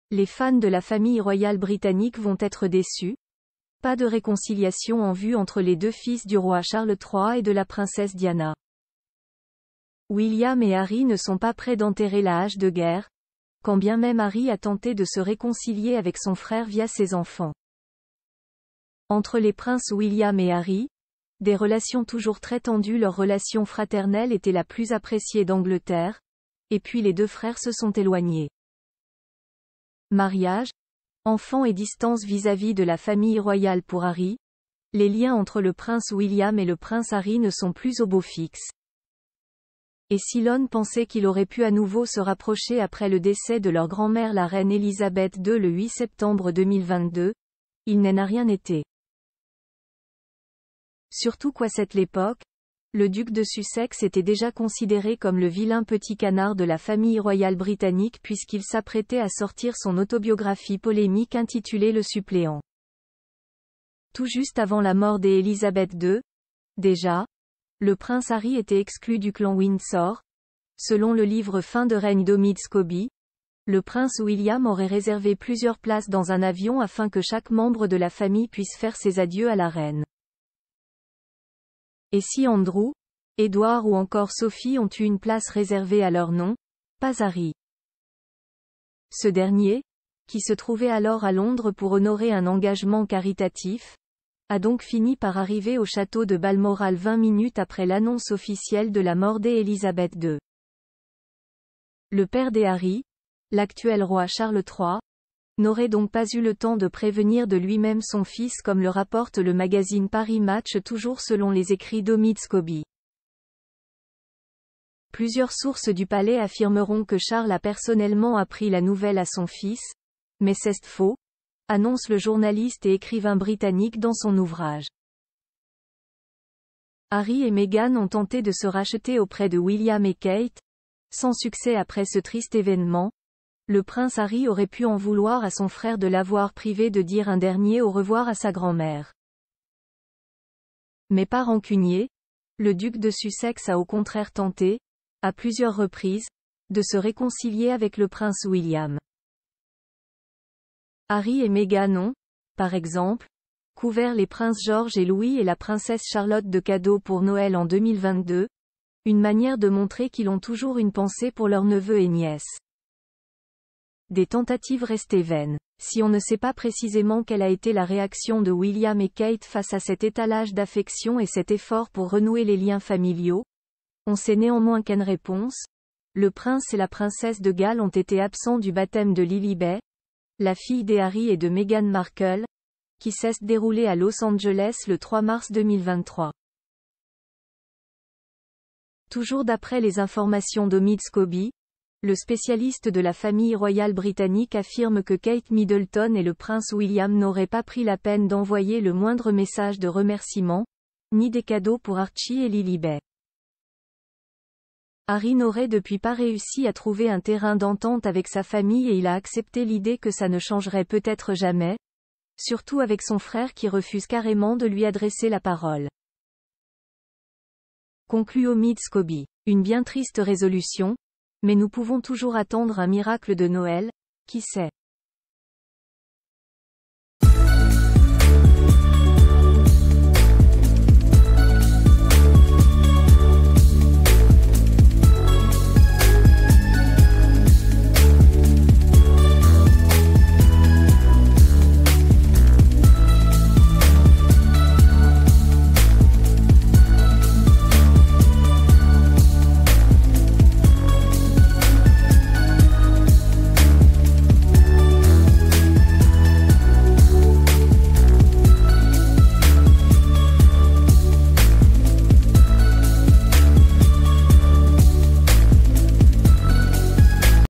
Harry et Meghan : ce geste envers leurs enfants qui n'a pas plu à Kate et William
Les fans de la famille royale britannique vont être déçus : pas de réconciliation en vue entre les deux fils du roi Charles III et de la princesse Diana. William et Harry ne sont pas prêts d'enterrer la hache de guerre, quand bien même Harry a tenté de se réconcilier avec son frère via ses enfants.   Entre les princes William et Harry, des relations toujours très tendues  Leur relation fraternelle était la plus appréciée d'Angleterre, et puis les deux frères se sont éloignés. Mariages, enfants ...